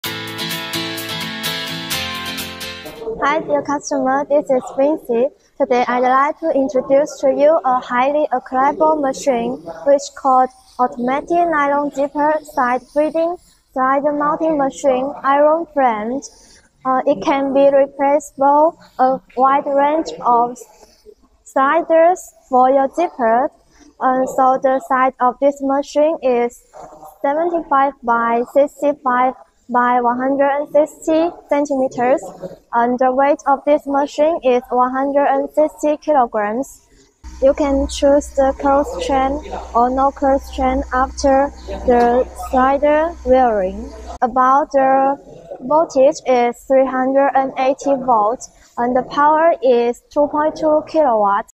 QLQ-SMM-4 Automatic Nylon Zipper Double Side Slider Mounting Machine (side mounting model)
It can be replaced with a wide range of sliders for your zippers.
You can chose the close chain or no close chain after
the slider wearing.